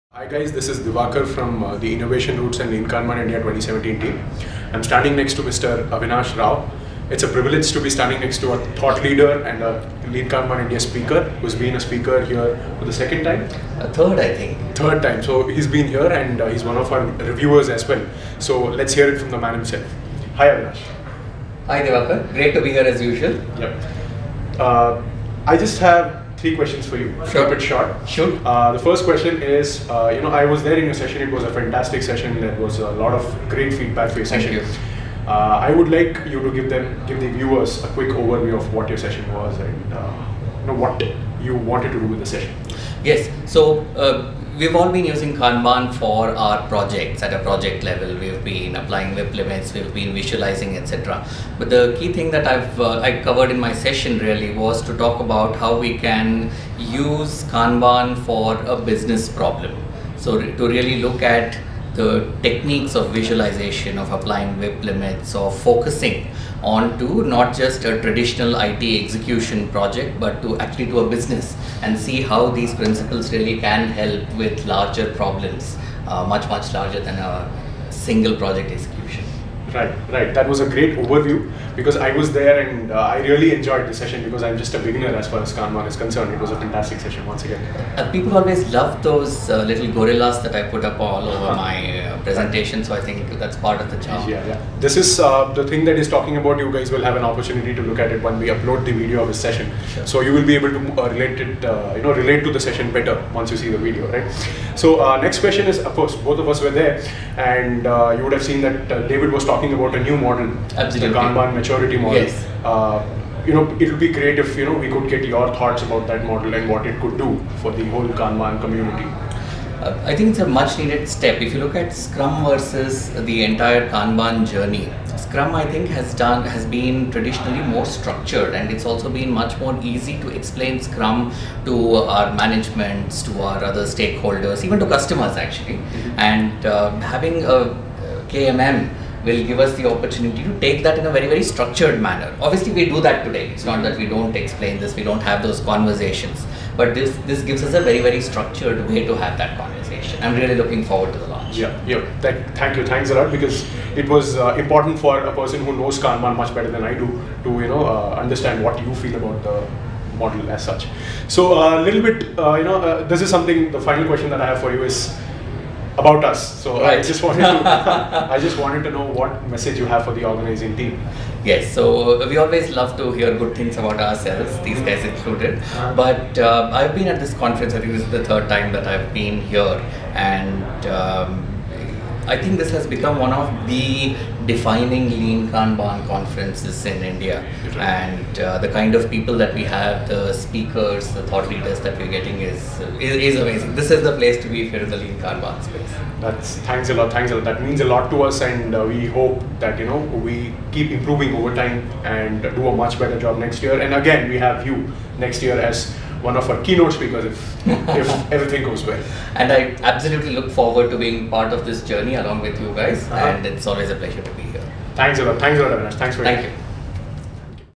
In Conversation with Avinash Rao | Lean Kanban India 2017 Conference
Lean Kanban India 2017 Conference has been a platform to further The Kanban Method, hereby referred to as Kanban, and its adoption. Kanban has often been referred to as the "Alternate path to Agility".